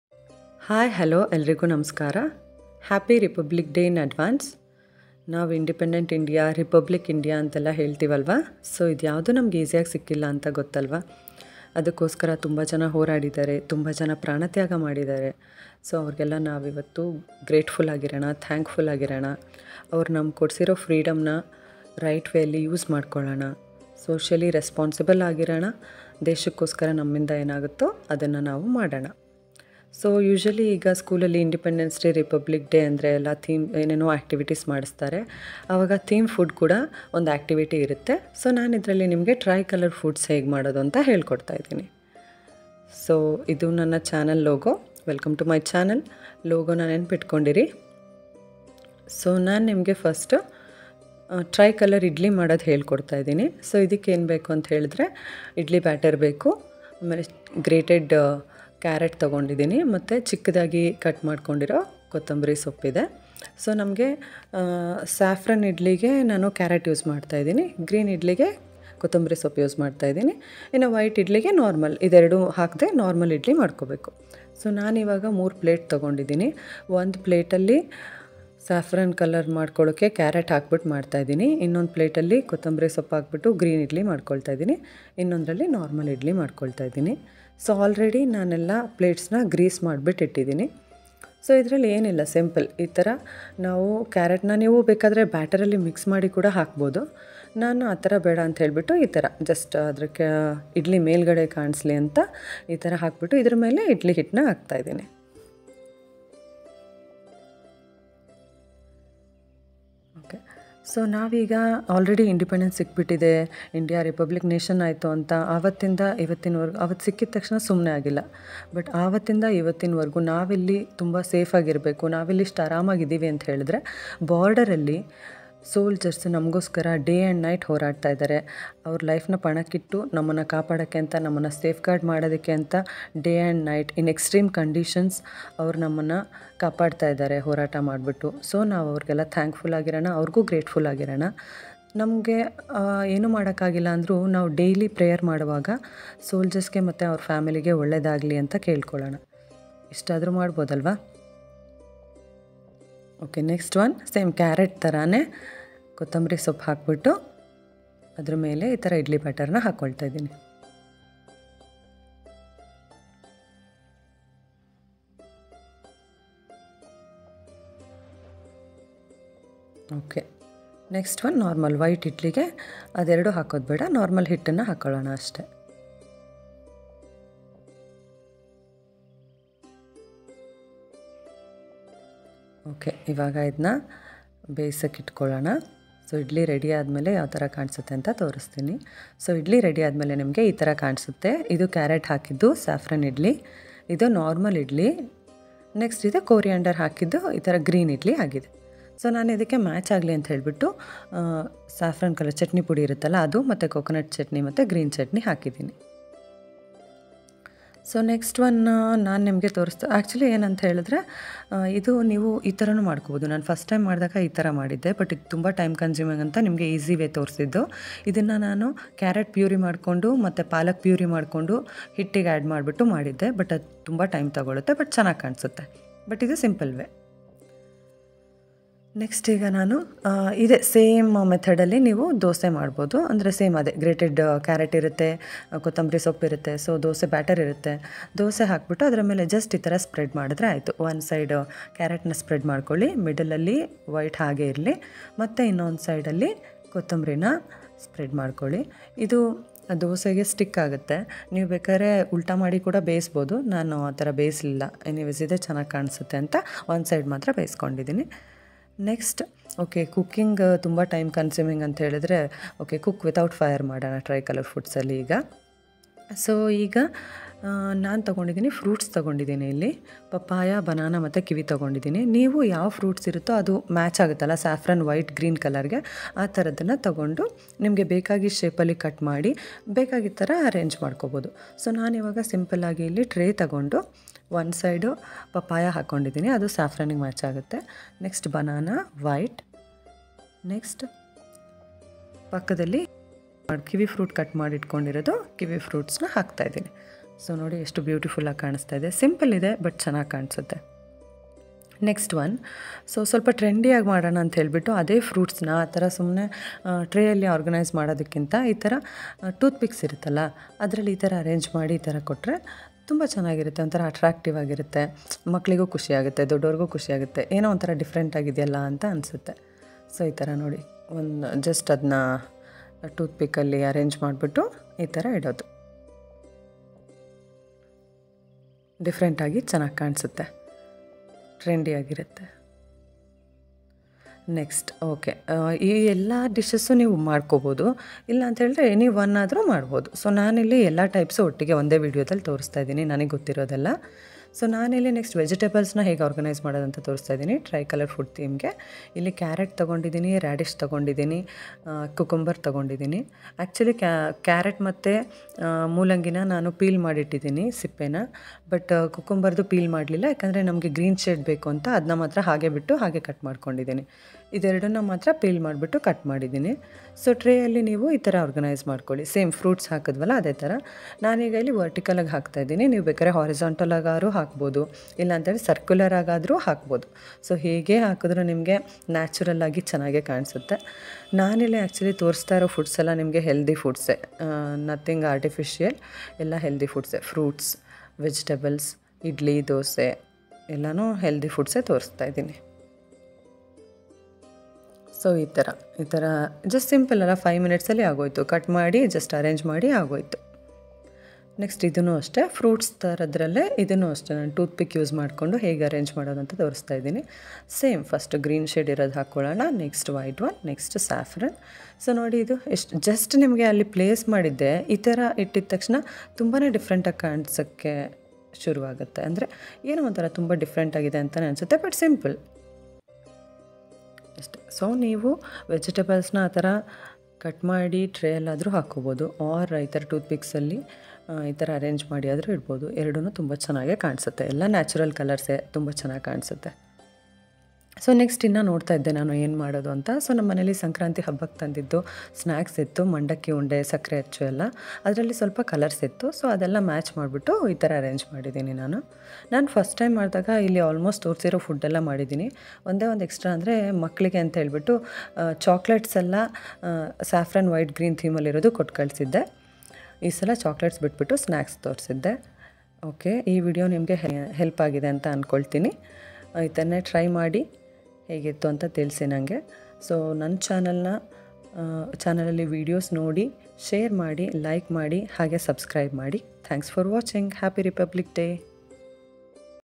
Republic/Independence Day food theme ideas | Tricolour dishes Recipes | Tiranga food ideas
Hello Everyone..

In this video I am showing how to make tricolour foods. Hope you like the video.